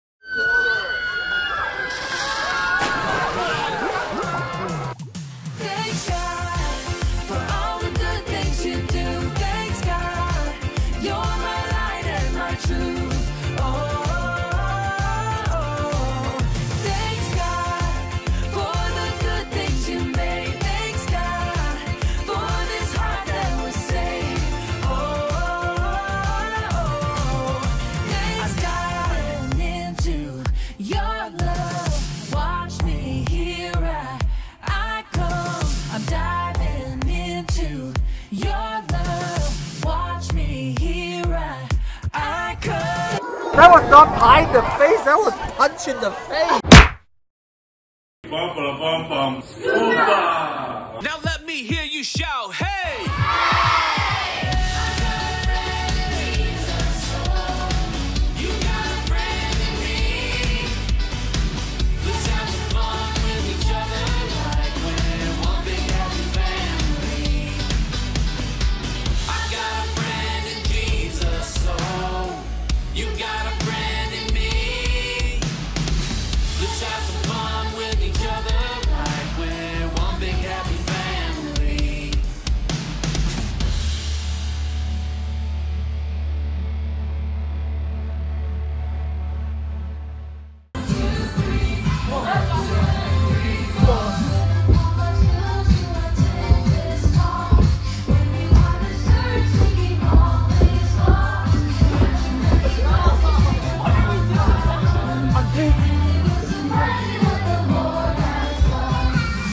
Crosspoint VBS Highlights: A Week of Joy and Adventure
🌞 A Week of Joy and Adventure🌞

Last week, our Vacation Bible School was a blast! From the moment the kids arrived, the excitement was palpable, and every day was packed with fun, learning, and unforgettable memories.

✨ Don't Miss Out Next Year! ✨

We can't wait to welcome even more happy campers next year.

Come next year and be part of the magic! 🌟